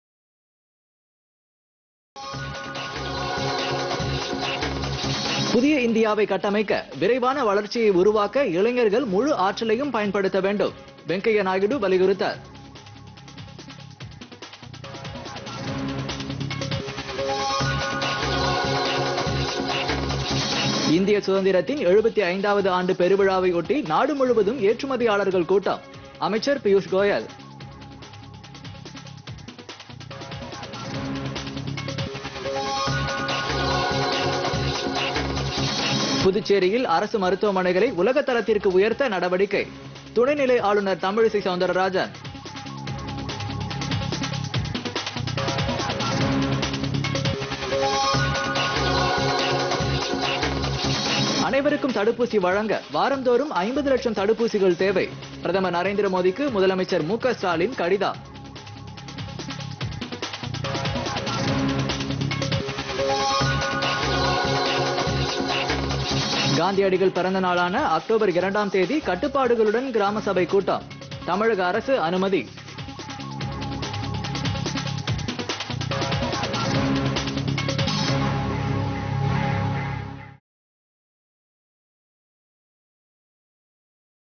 பொதிகை இரவு 7.00 மணி தலைப்புச் செய்திகள் [20.09.2021]
1) புதிய இந்தியாவை கட்டமைக்க,  விரைவான வளர்ச்சியை உருவாக்க இளைஞர்கள் முழு ஆற்றலையும் பயன்படுத்த வேண்டும்- வெங்கய்ய நாயுடு வலியுறுத்தல்.

2)  இந்திய சுதந்திரத்தின் 75 ஆவது ஆண்டு பெருவிழாவை ஒட்டி நாடு முழுவதும் ஏற்றுமதியாளர்கள் கூட்டம் - அமைச்சர் பியூஷ்கோயல்

3)  புதுச்சேரியில்  அரசு மருத்துவமனைகளை உலகத்தரத்திற்கு உயர்த்த நடவடிக்கை - துணை நிலை ஆளுநர் தமிழிசை செளந்தரராஜன்.

4) அனைவருக்கும் தடுப்பூசி வழங்க வாரந்தோறும் ஐம்பது லட்சம் தடுப்பூசிகள் தேவை - பிரதமர் நரேந்திர ‍மோதிக்கு முதலமைச்சர் மு க  ஸ்டாலின் கடிதம்.

5)   காந்தியடிகள் பிறந்தநாளான அக்டோபர் இரண்டாம் தேதி கட்டுப்பாடுகளுடன் கிராம சபைக் கூட்டம் - தமிழக அரசு அனுமதி.